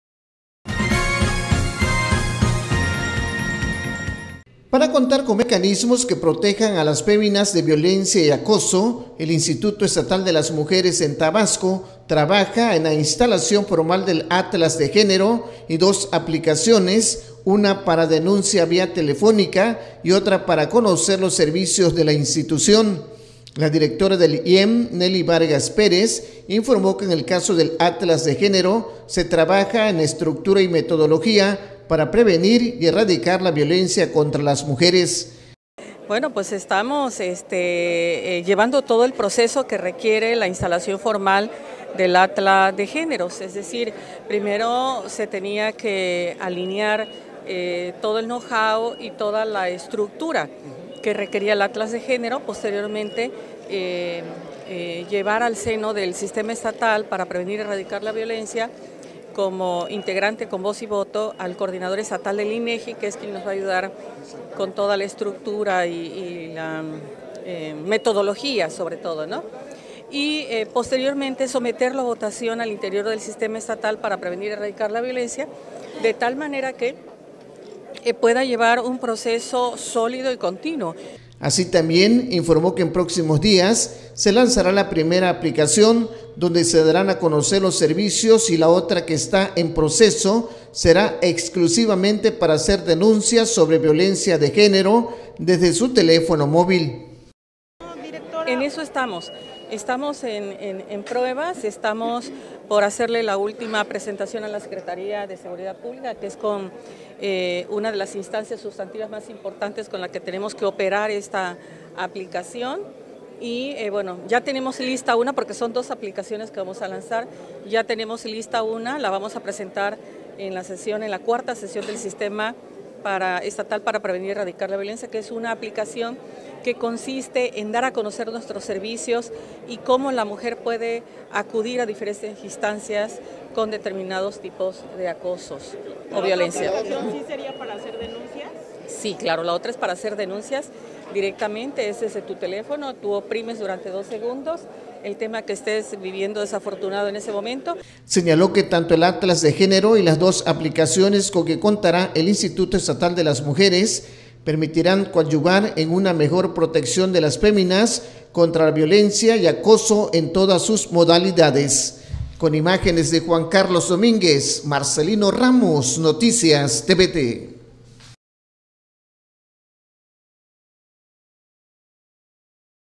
Contará IEM con atlas de género y aplicaciones para mujeres
Para contar con mecanismos que protejan a las féminas de violencia y acoso, el Instituto Estatal de las Mujeres en Tabasco, trabaja en la instalación formal del "Atlas de Género" y dos aplicaciones digitales, así lo da a conocer su directora